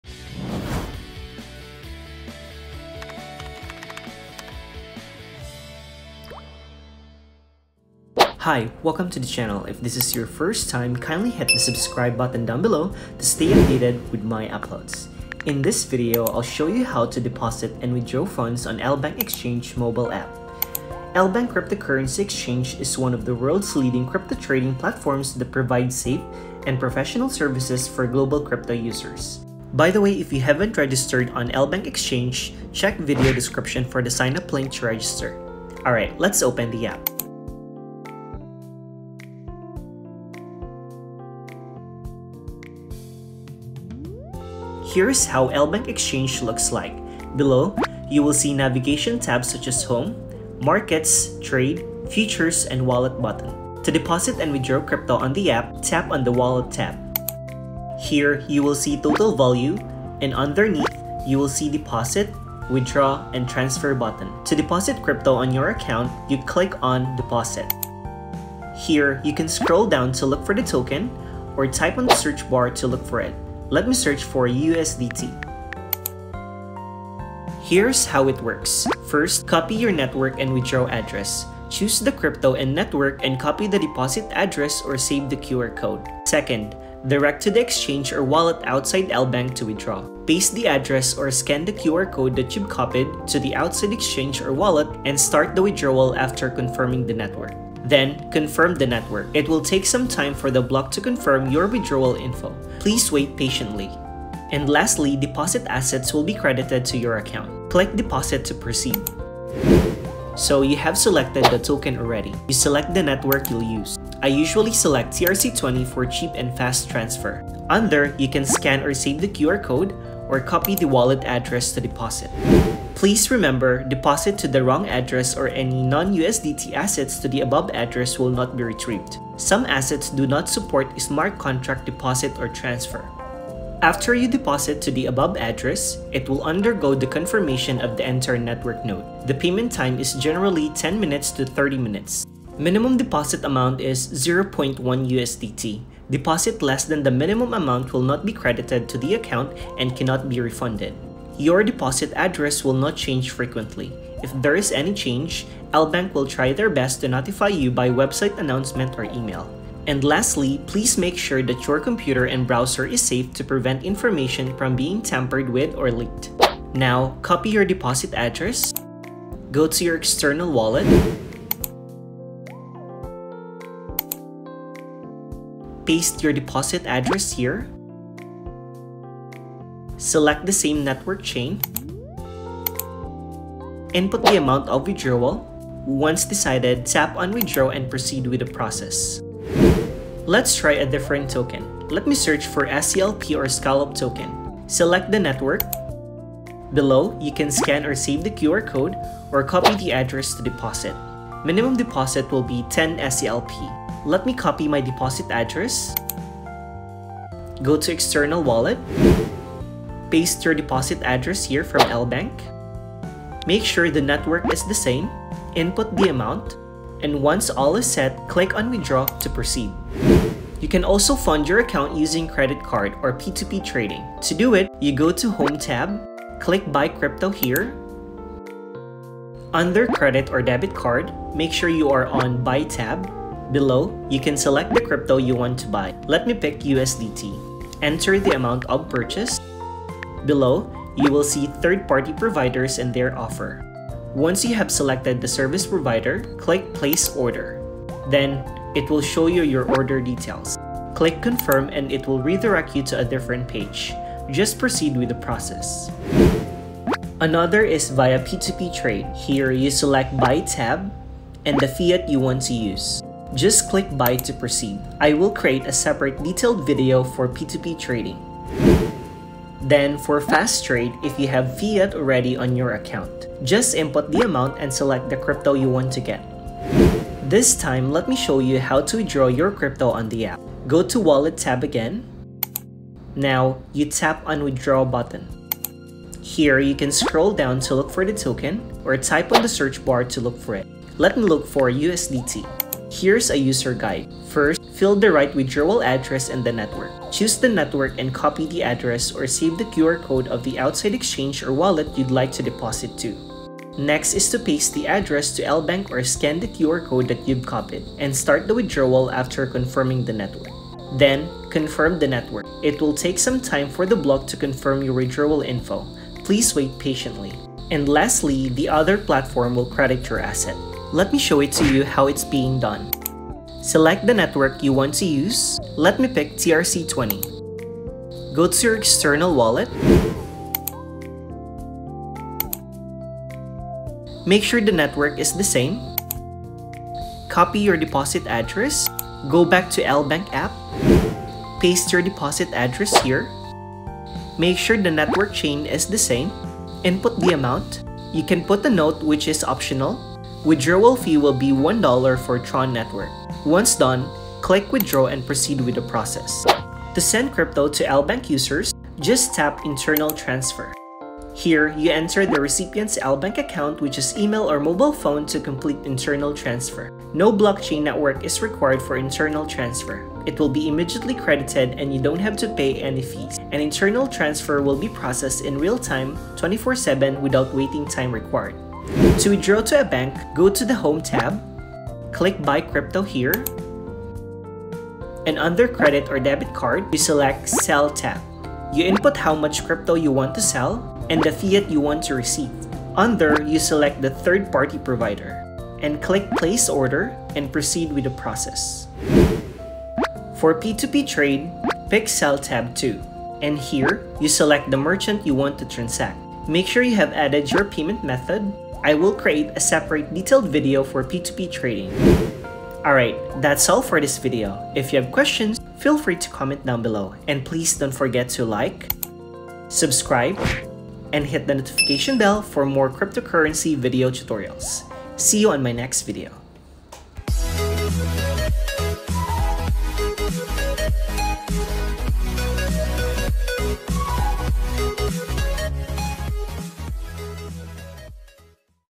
How to DEPOSIT or WITHDRAW on Lbank Exchange | Crypto App Tutorial
Here’s a video tutorial on how to deposit and withdraw funds on Lbank Exchange mobile app for beginners. Lbank is Hong Kong-based cryptocurrency exchange founded back in 2015 and is one of the world's leading crypto trading platforms providing safe and professional services for global crypto users.